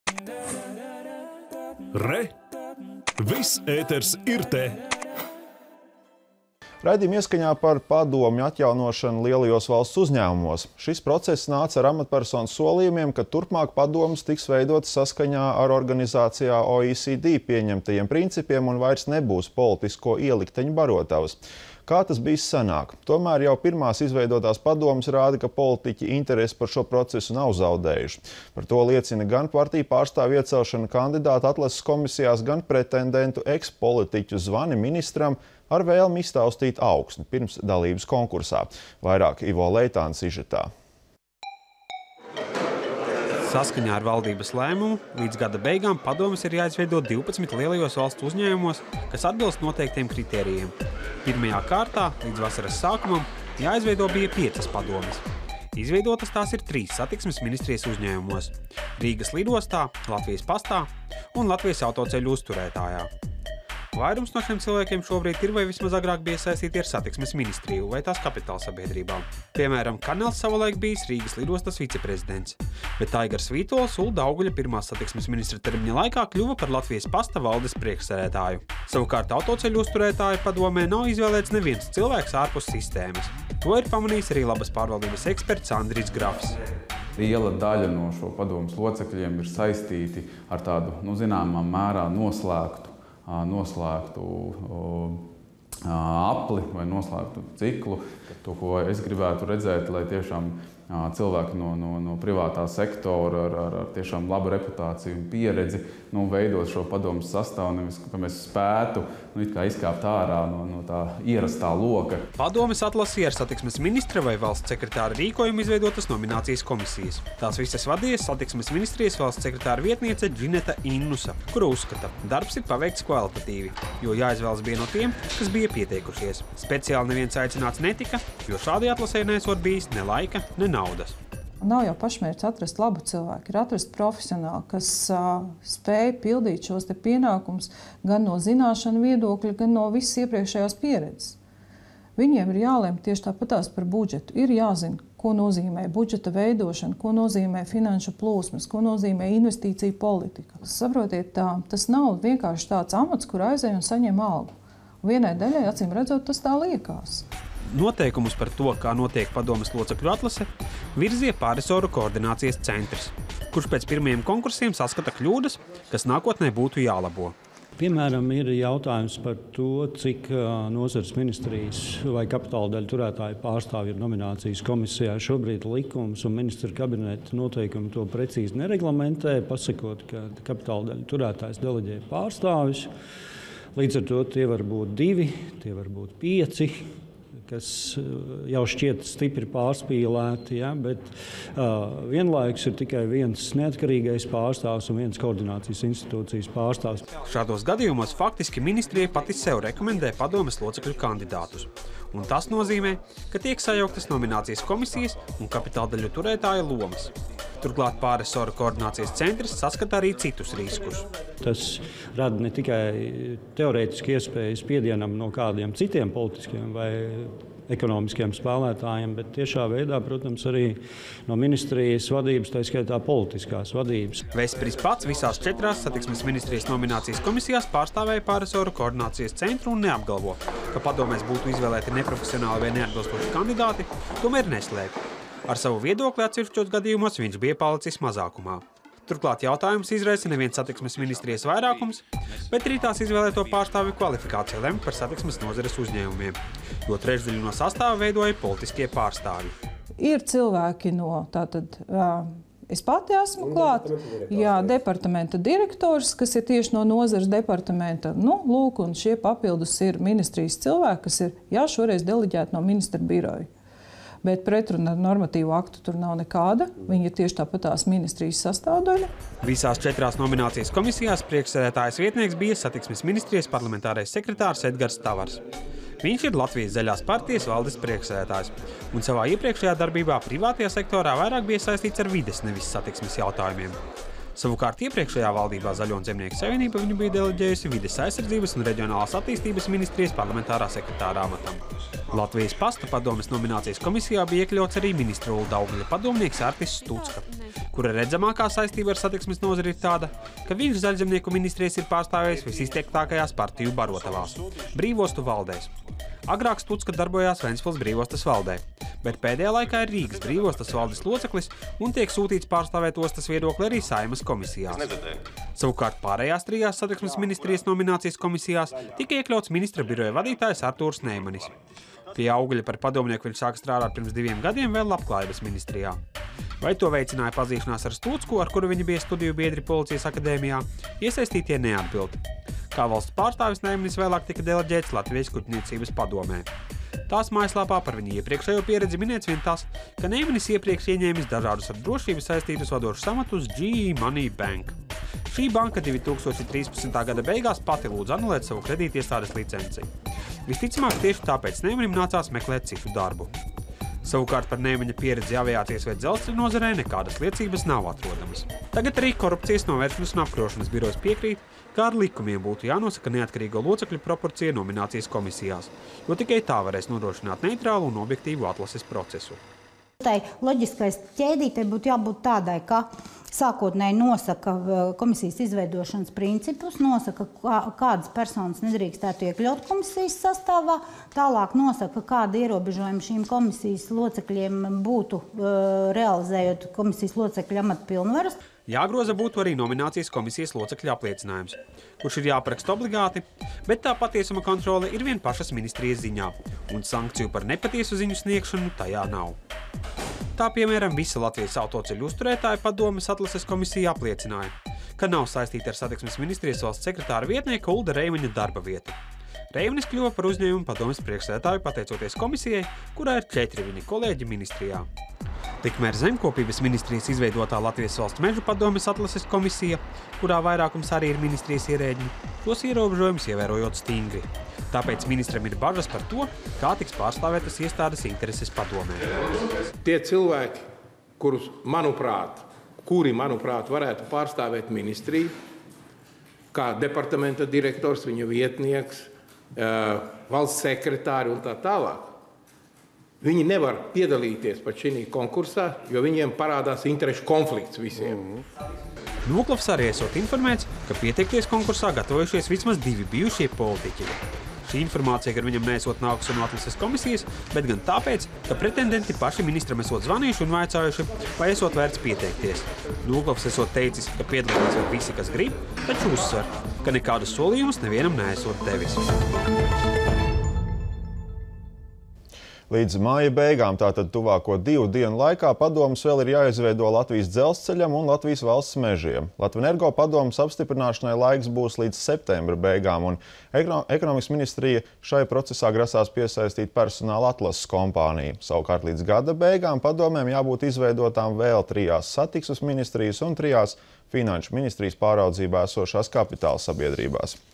Valsts uzņēmumu padomju izvēlē bez politiķiem joprojām neiztiek
Raidījuma ieskaņā par padomju atjaunošanu lielajos valsts uzņēmumos. Šis process nāca ar amatpersonu solījumiem, ka turpmāk padomes tiks veidotas saskaņā ar organizācijā OECD pieņemtajiem principiem un vairs nebūs politisko ielikteņu barotavas, kā tas bijis senāk. Tomēr jau pirmās izveidotās padomes rāda, ka politiķi interesi par šo procesu nav zaudējuši. Par to liecina gan partiju pārstāvju iecelšana kandidātu atlases komisijās, gan pretendentu-ekspolitiķu zvani ministram ar vēlmi iztaustīt augsni pirms dalības konkursā. Vairāk – Ivo Leitāna sižetā.